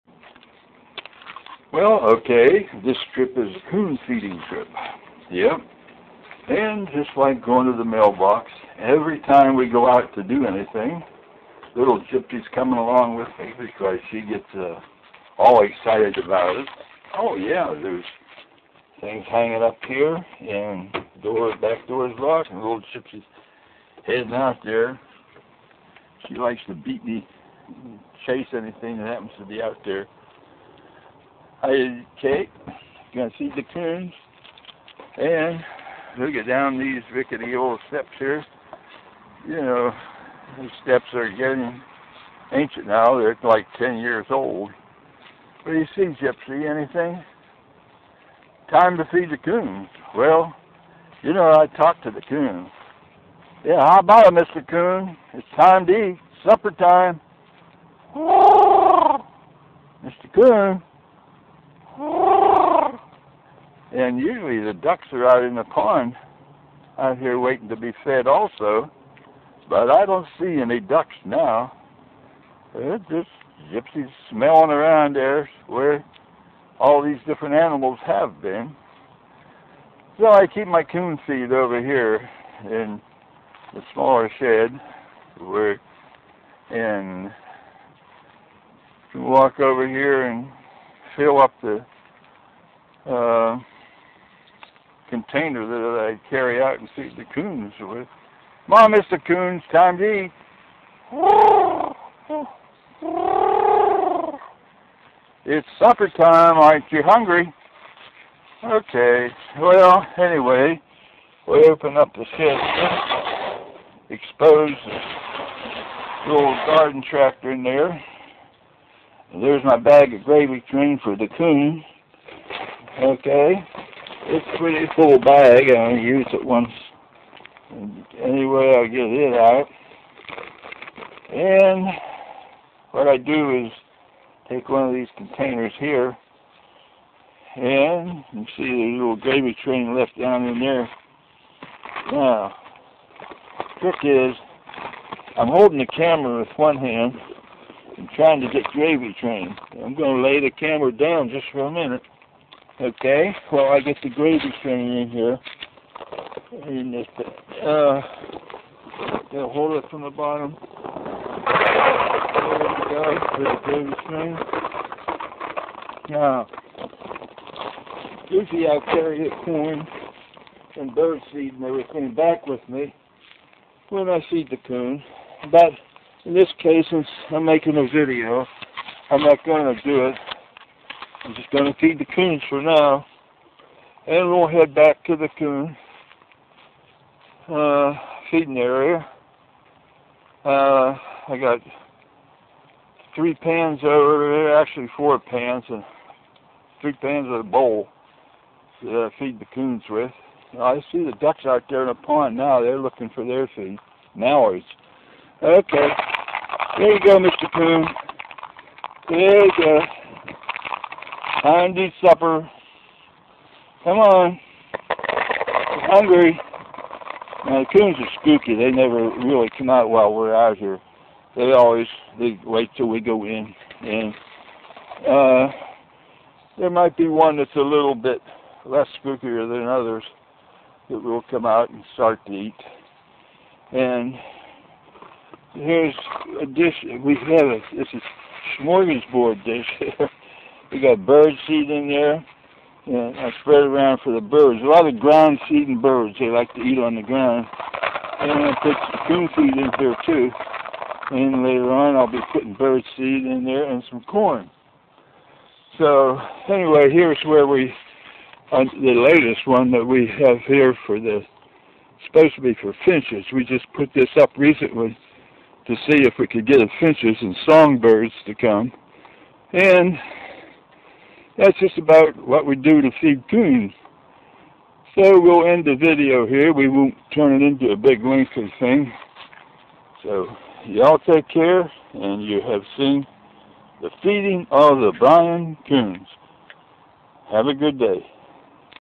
To feed the coon 2
At 4:30pm or there about it's time to feed the animals